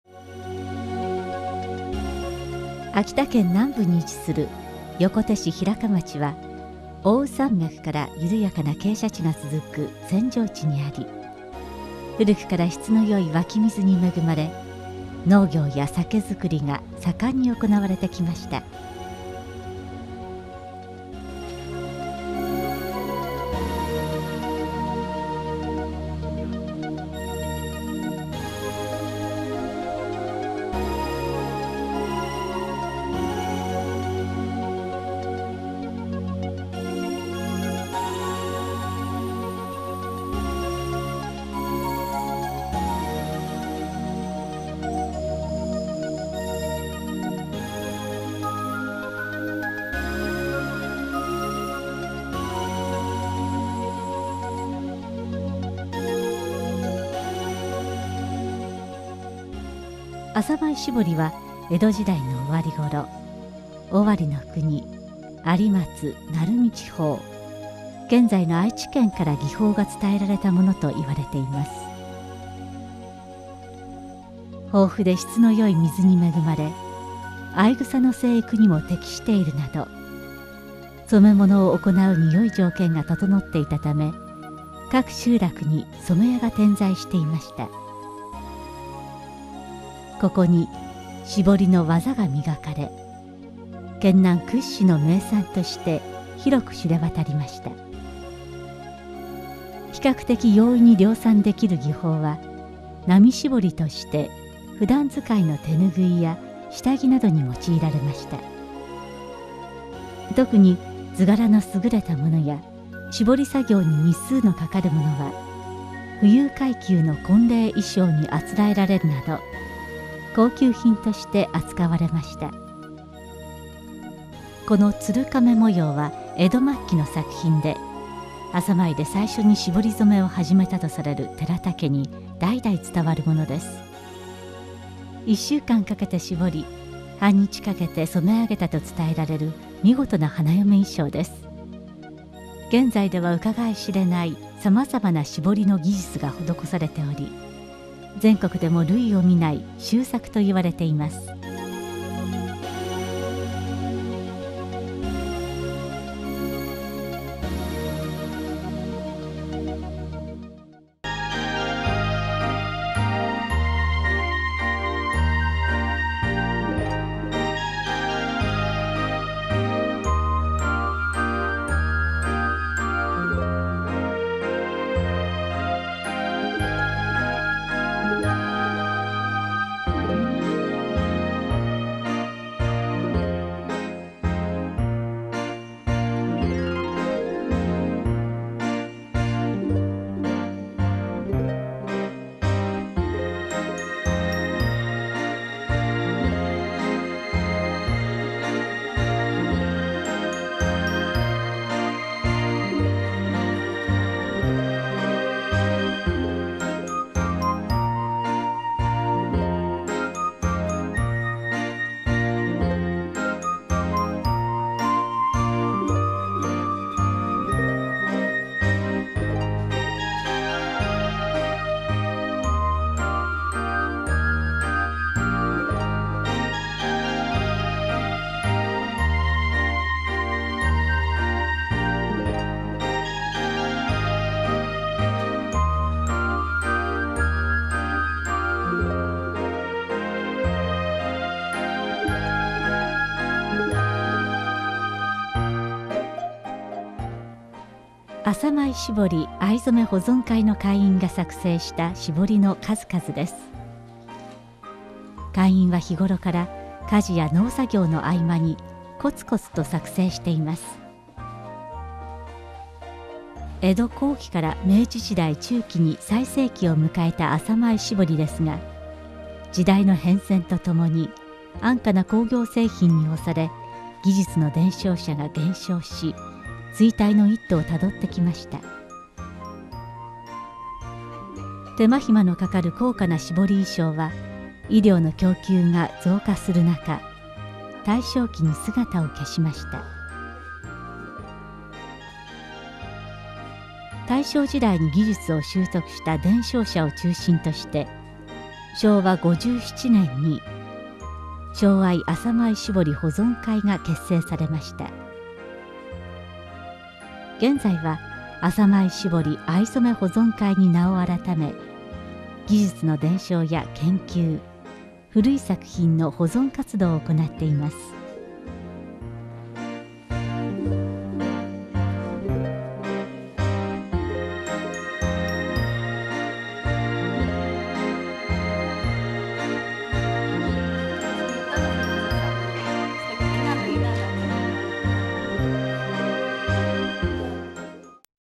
【浅舞絞り】受け継がれる伝統の匠（平鹿町浅舞）
平成２１年８月５日～９月２５日撮影、浅舞絞りは江戸時代後期から明治時代中期に盛んに生産されて、県南部の富裕階層の婚礼衣装として作られた高価なものでした。
　それから後、昭和57年に伝承者を中心に「正藍浅舞絞り保存会」が結成され、絞りの技術が活かされ現在に至っている。全て当スタジオで制作しました。